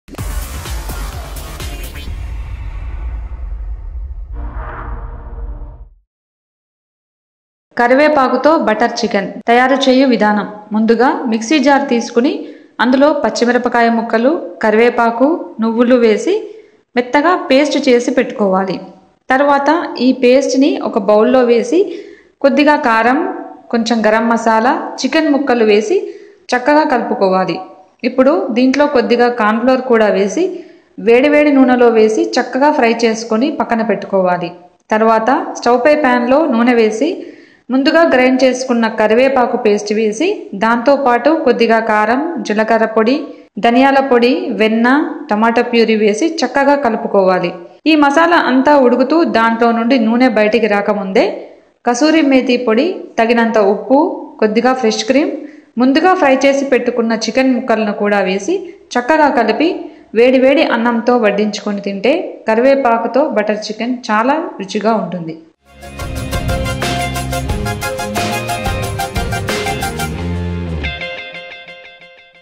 KARIVEPAKU THO BUTTER CHICKEN - In Telugu
Chicken cooked with curry leaves masala is a very tasty preparation from Andhra Pradesh.